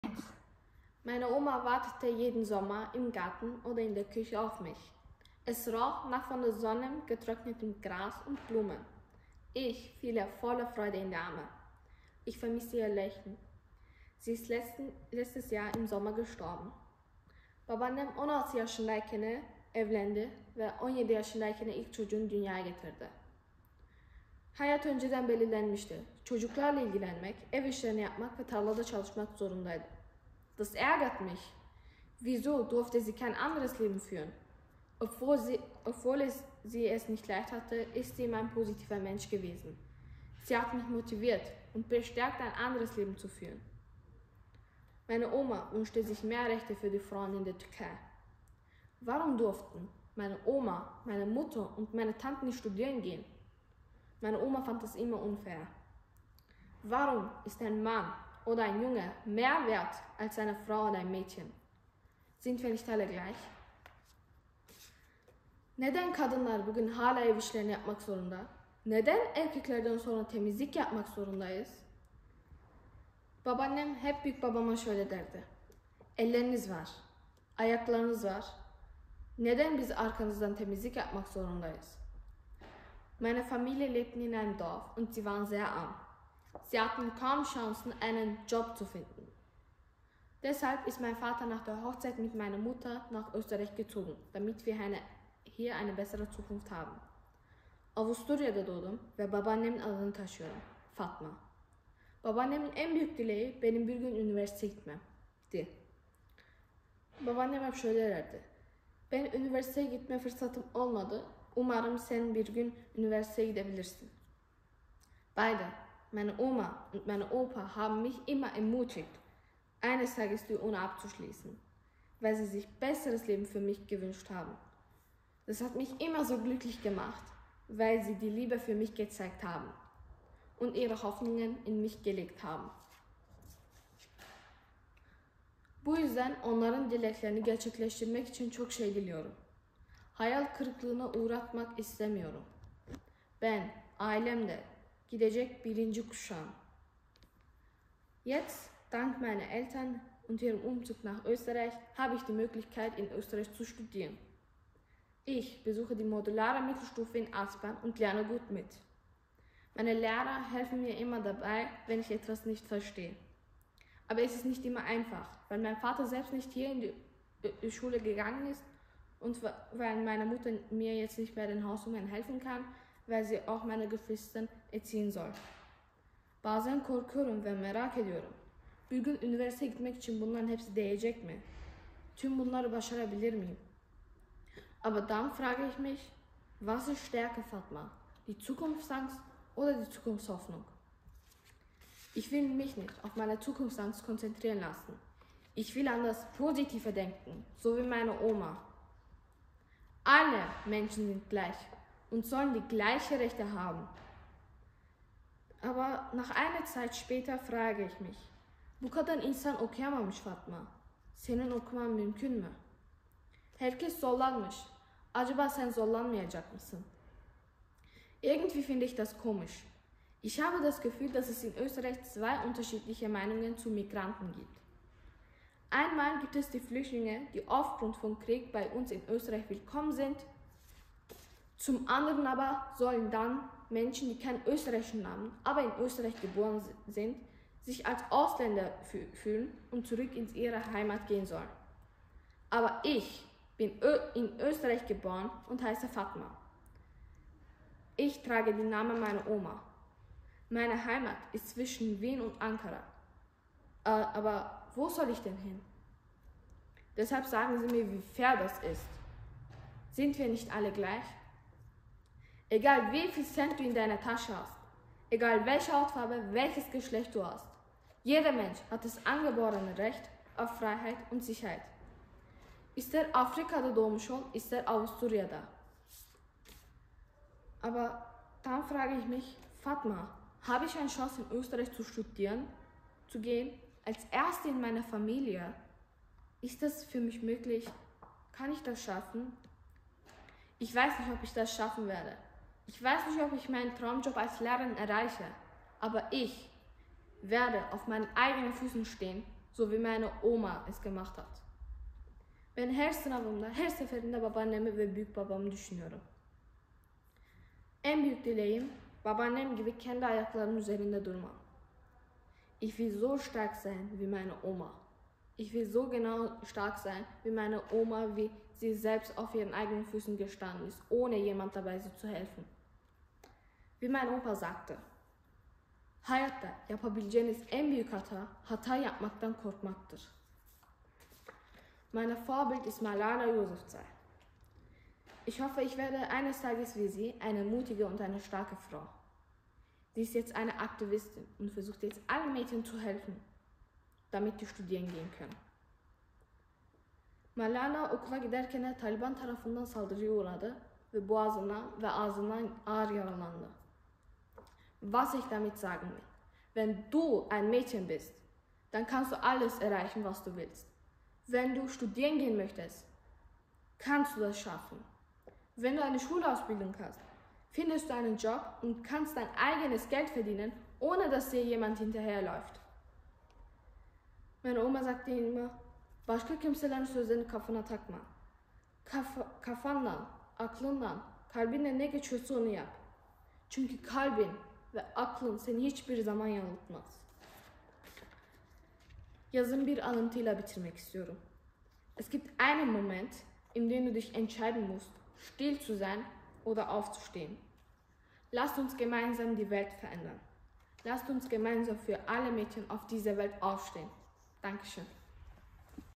Fatma Kocak - Türkisch & Deutsch - Hauptrunde (Kat. 1) von "SAG'S MULTI!" 2022/23
Bundesland: Wien
Schule: Modulare Mittelstufe Aspern Wien 22

Dieses Video ist eine Rede zur digitalen Teilnahme an der Hauptrunde des österreichweiten mehrsprachigen Redewettbewerbs "SAG'S MULTI!" im 14. Durchgang im Schuljahr 2022/23.

Das besondere an "SAG’S MULTI!" ist, dass in der Rede zwischen Deutsch und der Erstsprache oder Deutsch und einer erlernten Fremdsprache mehrmals gewechselt werden muss. Damit bietet der Bewerb jungen Menschen eine Plattform ihre Gedanken in Verbindung mit ihrer Mehrsprachigkeit und Internationalität vor Publikum zur präsentieren.